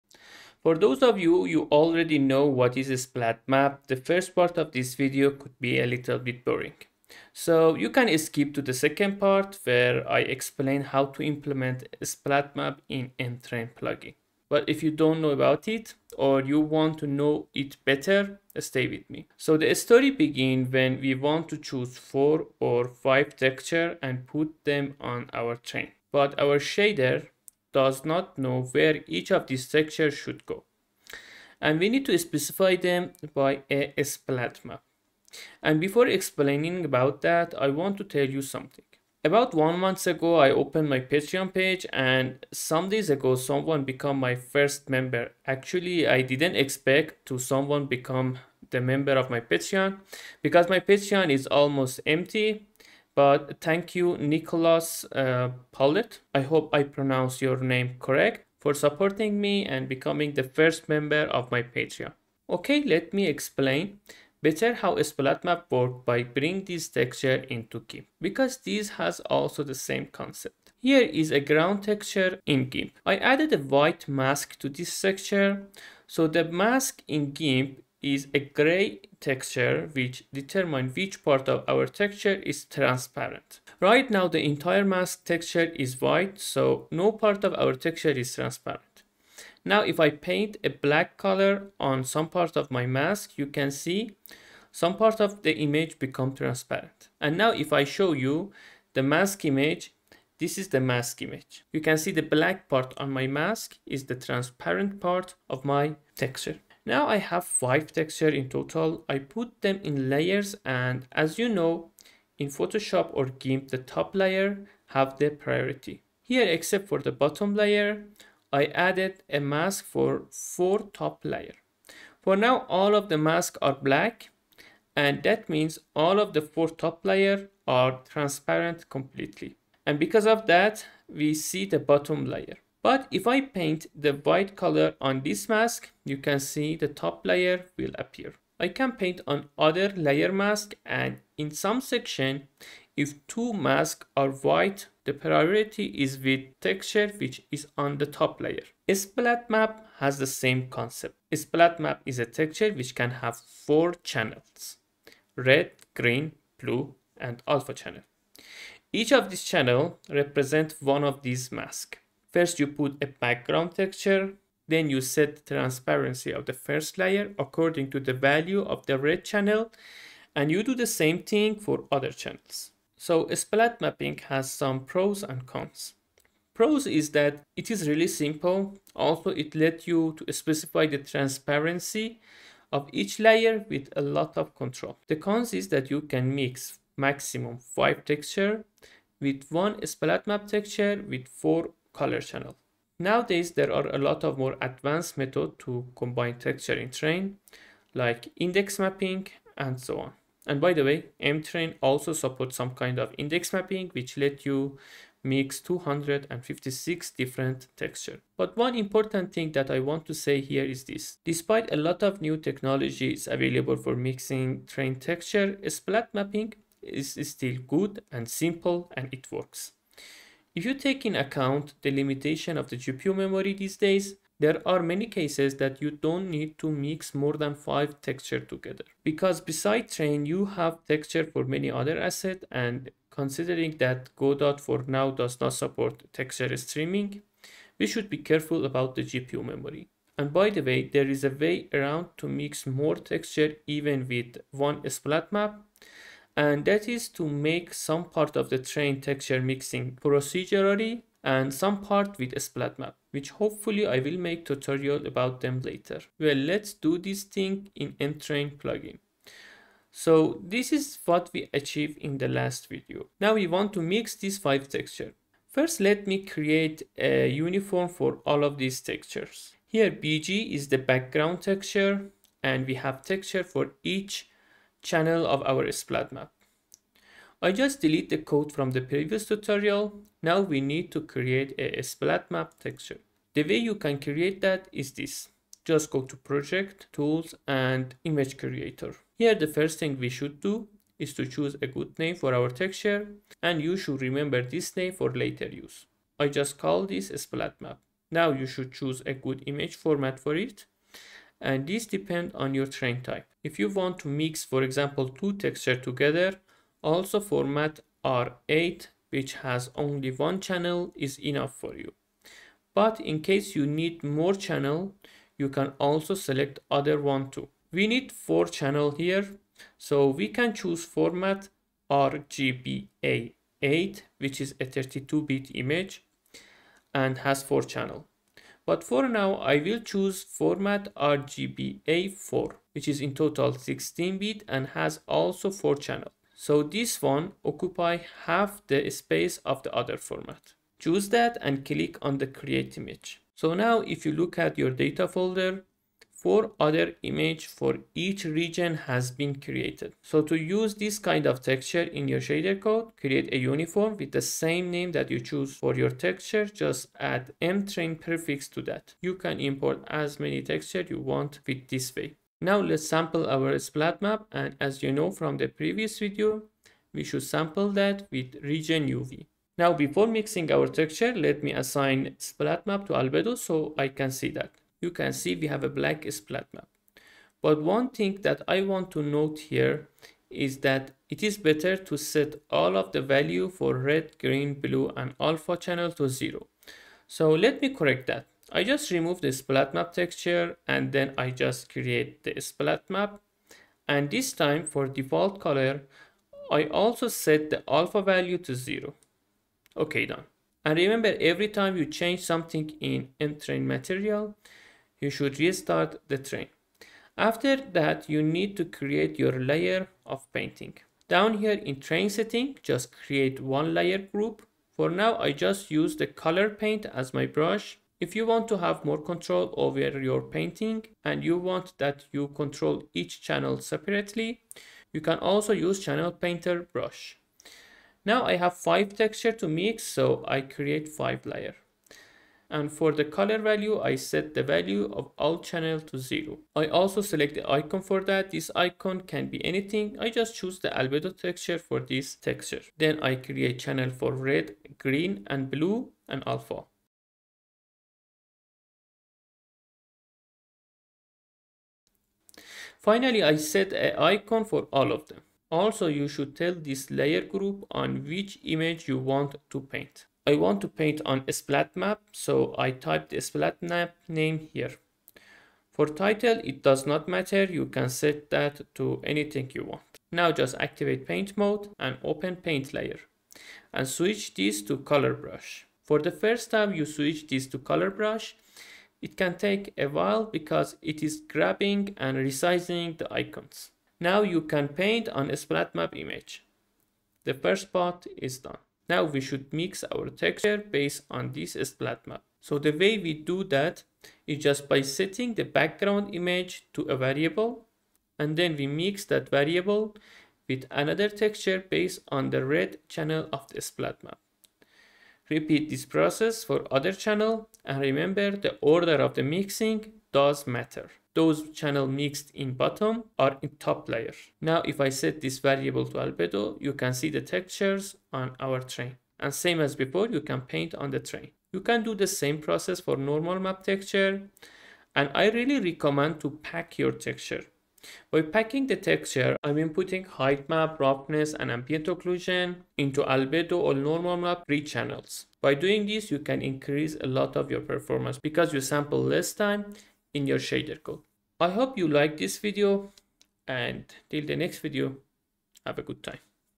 Splatmap in M Terrain - Godot engine
Splatmap is one of the classical way to put texture on terrain, in this video I am going to speak about the base concept of Splatmap, and how we can implement that in shader code

00:00-intro
00:36-my supporter
01:01-splatmap concept
04:16-splatmap in M Terrain